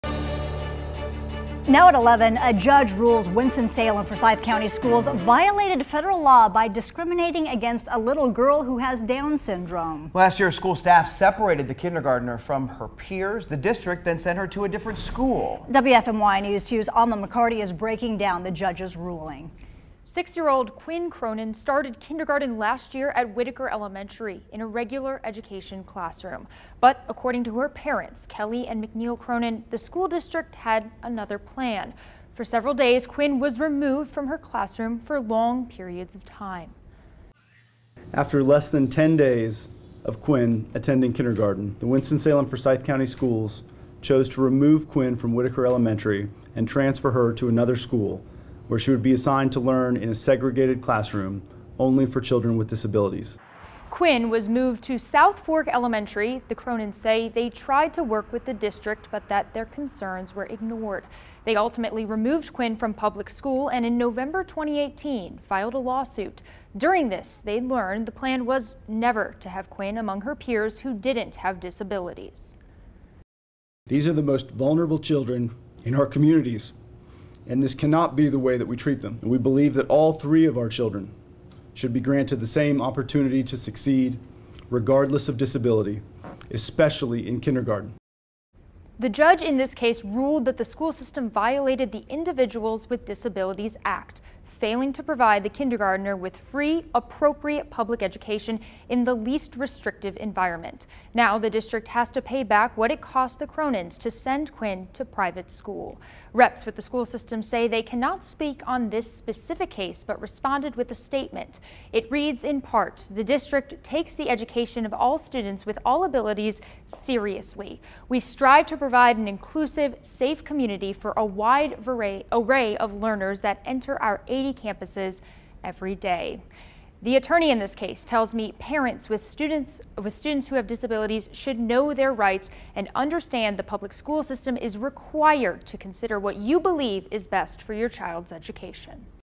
Winston-Salem Forsyth County Schools Violated Federal Law When It Moved Student: Judge
Last year, school staff separated the kindergartener from her peers. The district then sent her to a different school.